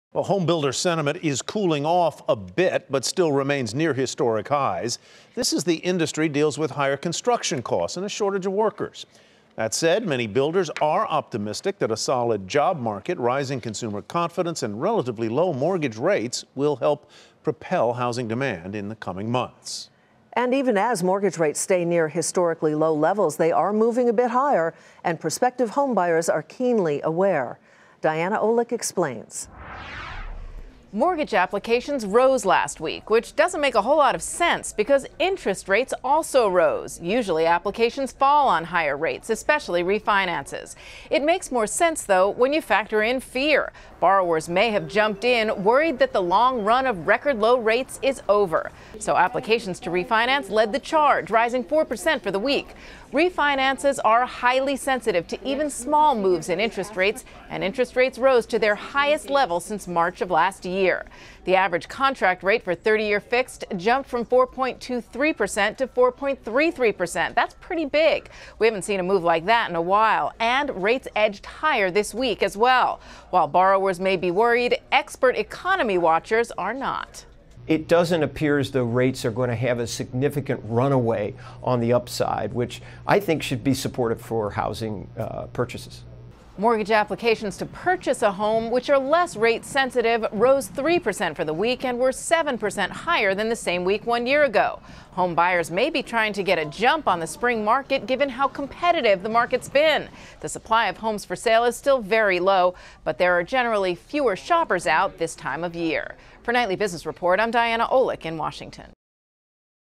Mortgage rates on the rise in 2018
Diana Olick takes a look at the rise in mortgage rates this year and what it could mean for the Spring housing market.